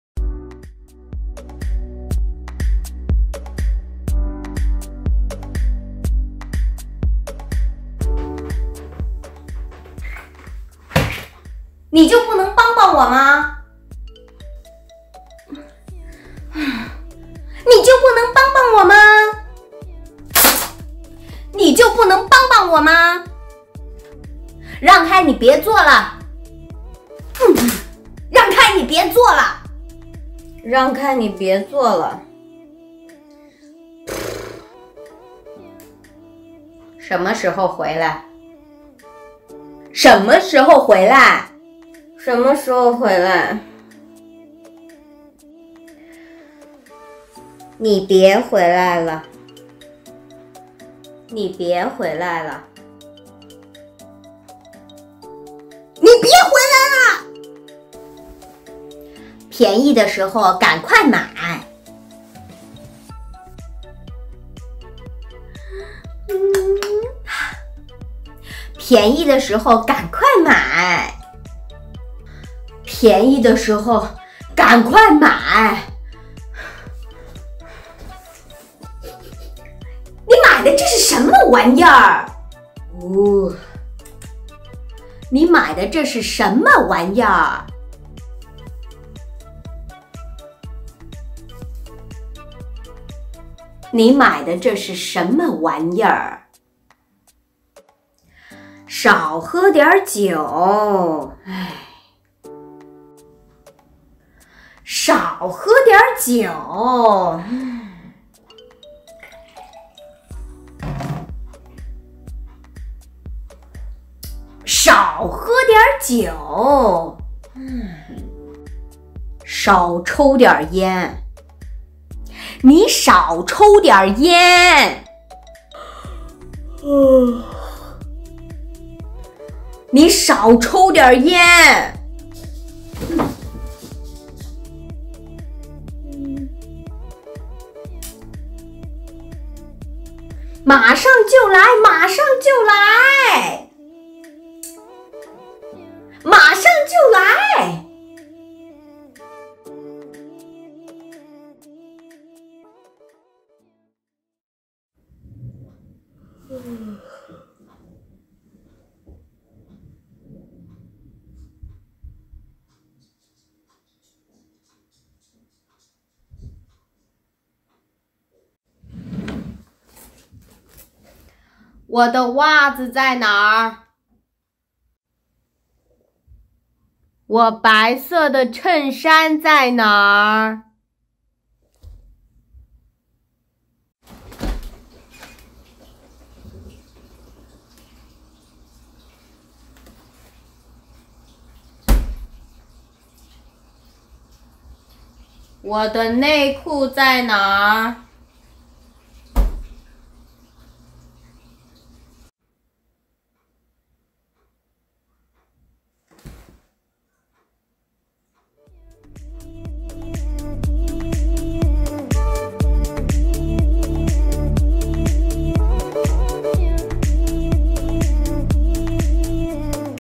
재미있는 중국어 아내 VS 남편 상황극
Nǐ jiù bùnéng bāng bāng wǒ ma
你就不能帮帮我吗
 나 좀 도와줄 수 있어?

Ràng kāi, nǐ bié zuòle

  让开，你别做了

    비켜. 하지마

Shénme shíhòu huílái

什么时候回来

언제와

Nǐ bié huíláile

 你别回来了

오지마

Piányí de shíhòu gǎnkuài mǎi!

便宜的时候赶快买！

쌀 때  빨리 사

Nǐ mǎi de zhè shì shénme wányì er?

 你买的这是什么玩意儿？

이게뭐야 도대체 뭘 산거야

Shǎo hē diǎn jiǔ

少喝点酒

술 그만 마셔

Shǎo chōu diǎn yān

少抽点烟

담배 그만 피워

Mǎshàng jiu lái mǎshàng jiu lái

马上就来  马上就来

바로 갈게

Wǒ de wàzi zài nǎ

我的袜子在哪

나 양말 어딨어?

Wǒ de nèikù zài nǎ

我的内裤在哪

내 팬티 어딨어?

Wǒ dì nà jiàn báisè chènshān zài nǎ?

我的那件白色衬衫在哪？

내 흰색 셔츠 어딨어?

Nǐ zài nǎ?

你在哪？

너 어디야?